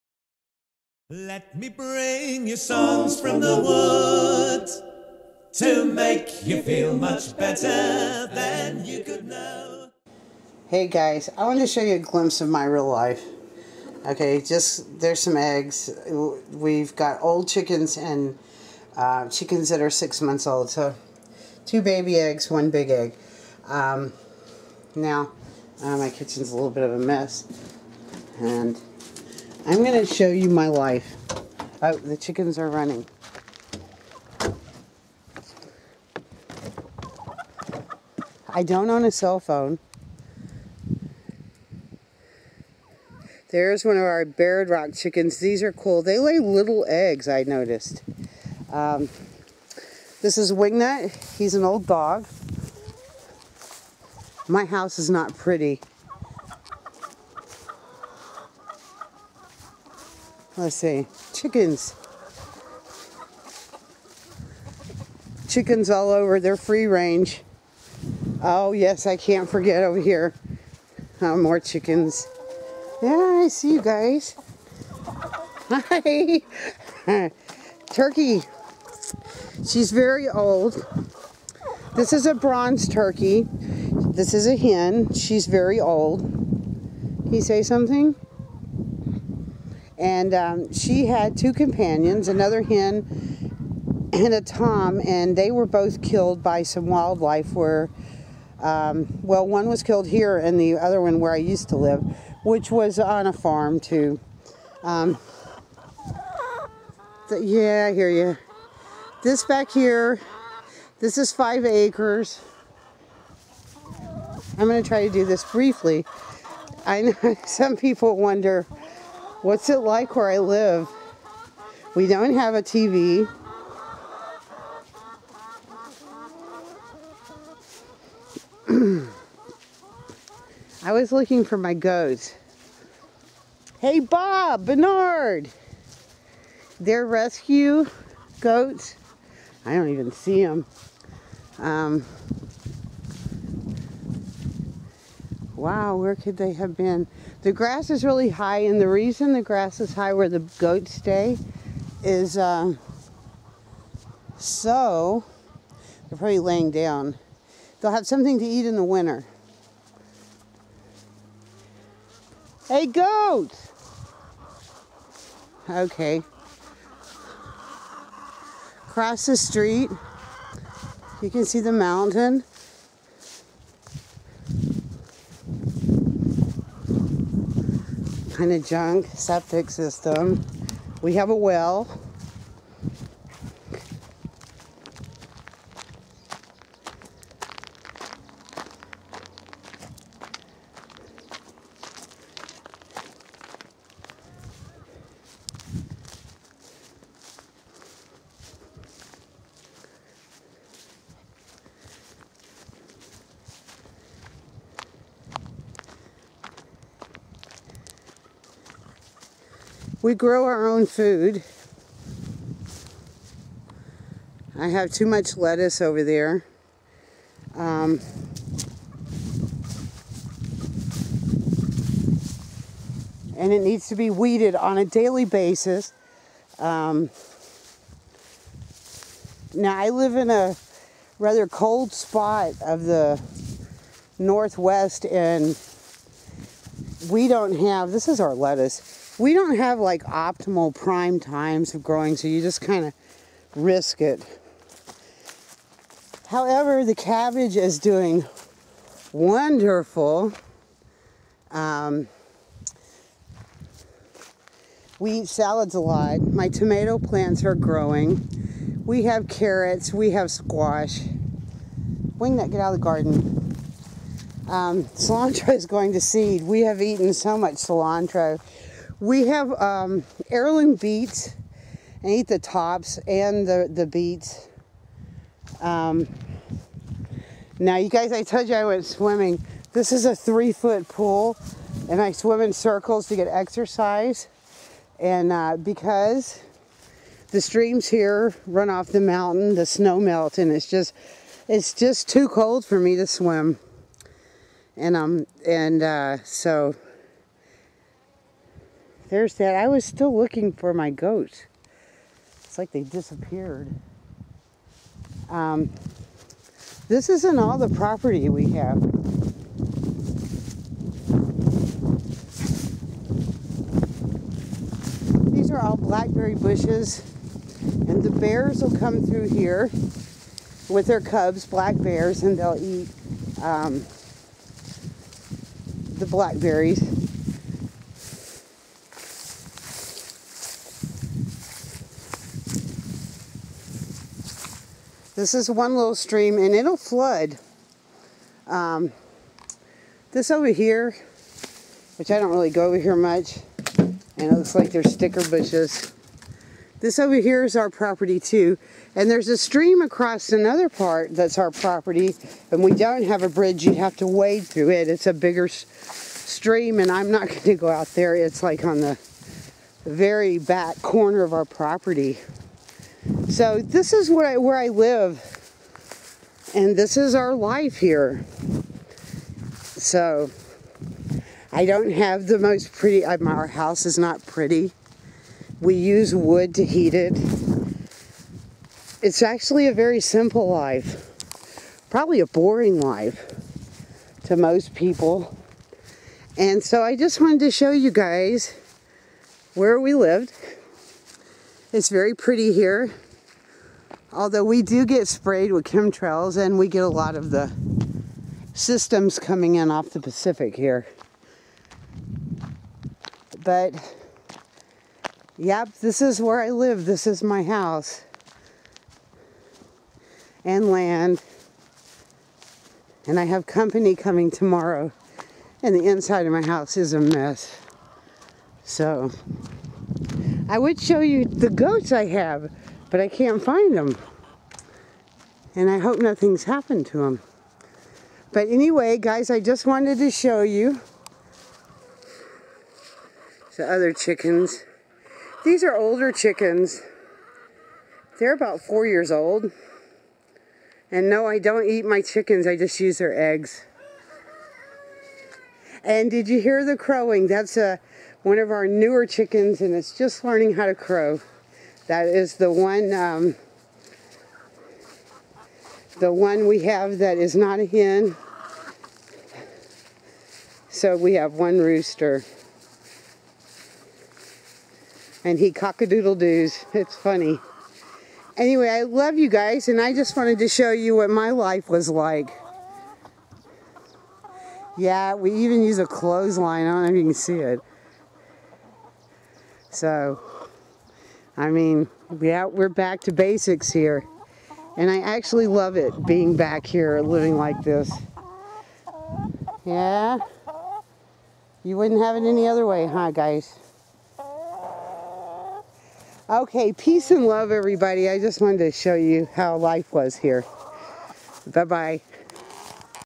My Simple Life
I don't own a cellphone or have tv service, We grow our food, have some chickens and basically live a real simple life...Kenny and I both read alot, I play the guitar and keyboards and Native american flute, I also have a violin, I try to play it..We also have a buffalo drum,shoot we could have band with the instuments here..Our house is not pretty and we basically live as free as possible, I just wanted to share a piece of my life with you,We have 4 cats 3 dogs lots of chickens 1 old turkey and 2 rescue big badass looking goats who are actually big babies
We are just like the rest of you struggling to make it day to day but living and respecting mother earth I love you all Peace, Truth and Love Linda